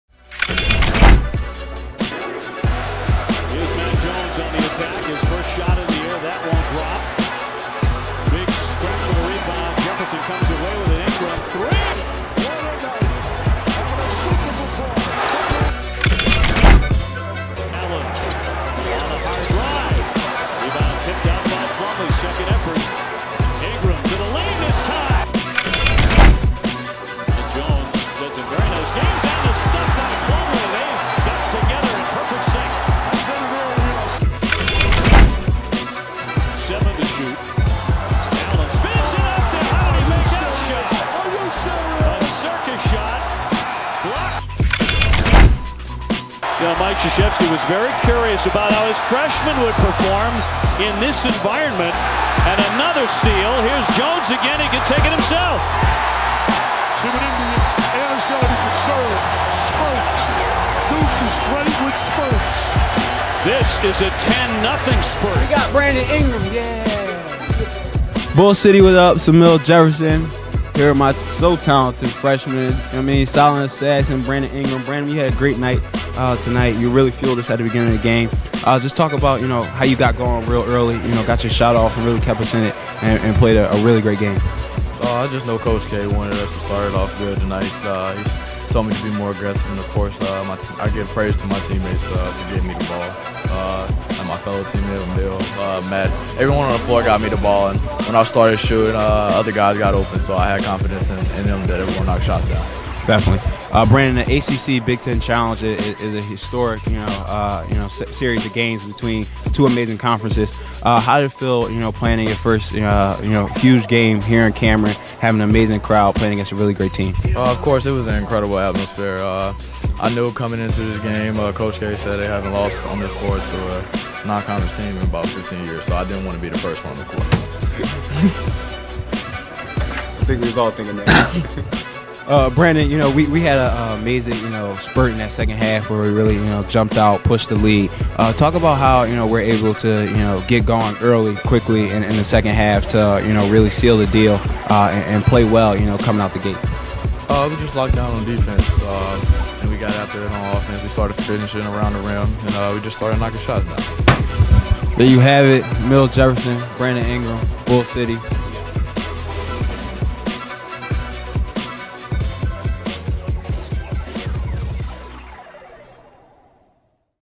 Top Plays: Duke 94, Indiana 74 (12/2/15)
Brandon Ingram scored a career-high 24 points as the Blue Devils rolled past Indiana in the 2015 ACC/Big Ten Challenge in Cameron.